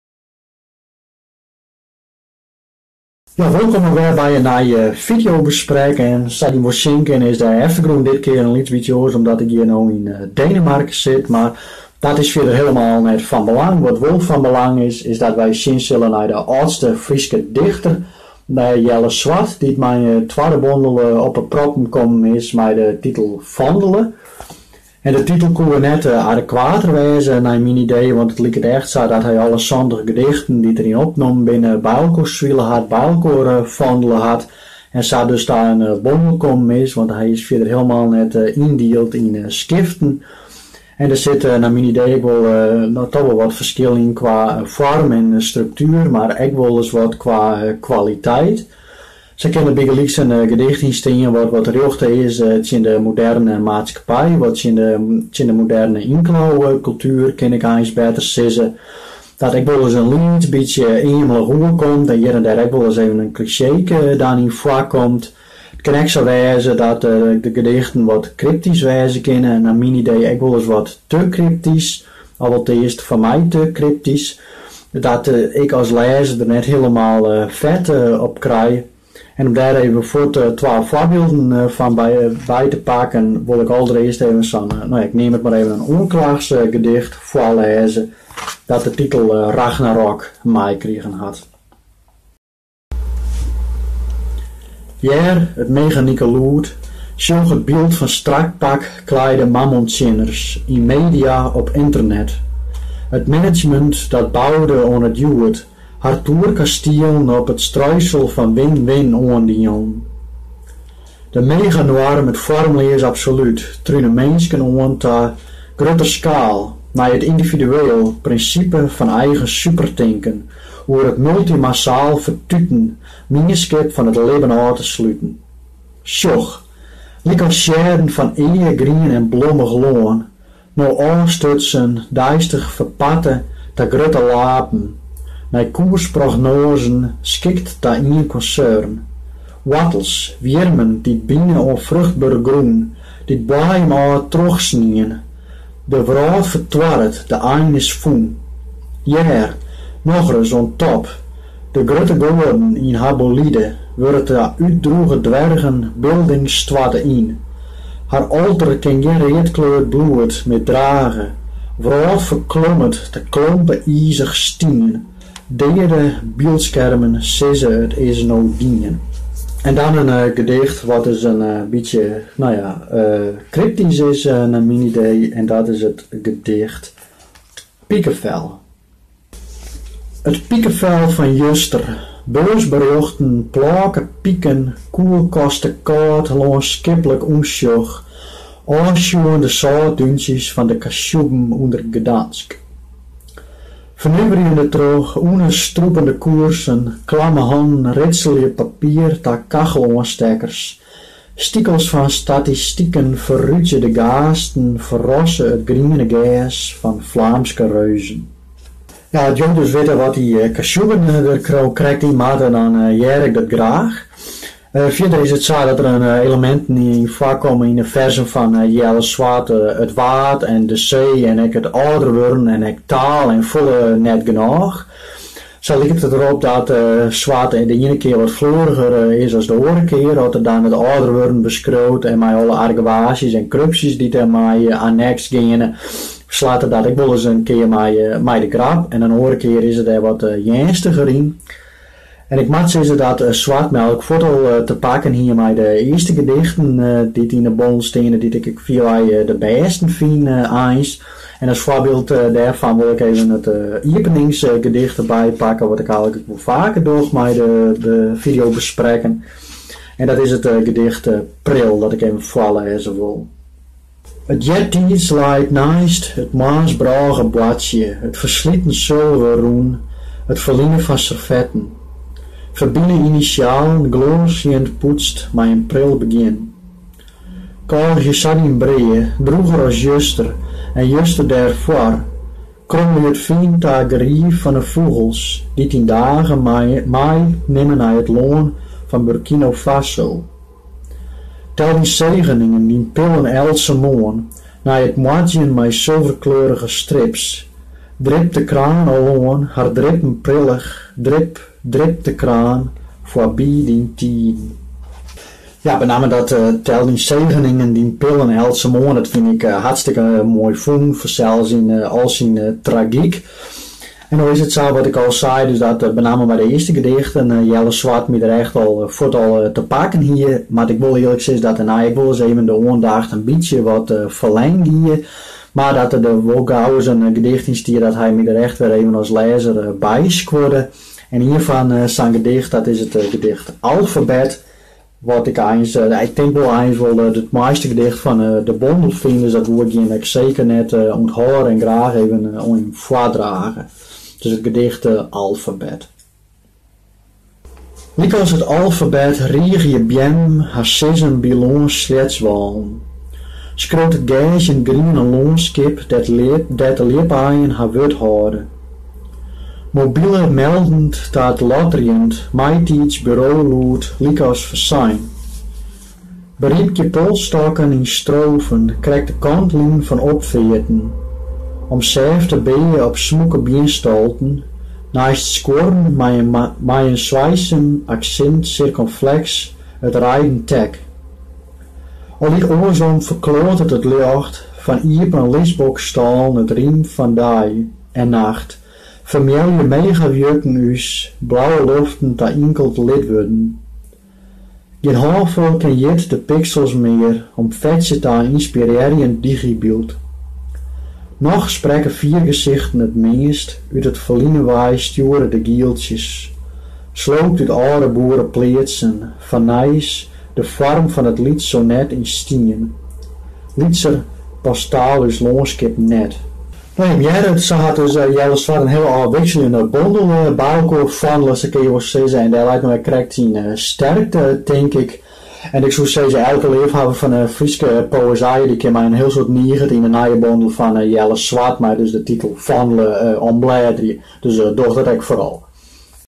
Fideobesprek 'Fandele' fan Jelle Zwart
Geart Tigchelaar besprekt de dichtbondel 'Fandele' fan Jelle Zwart.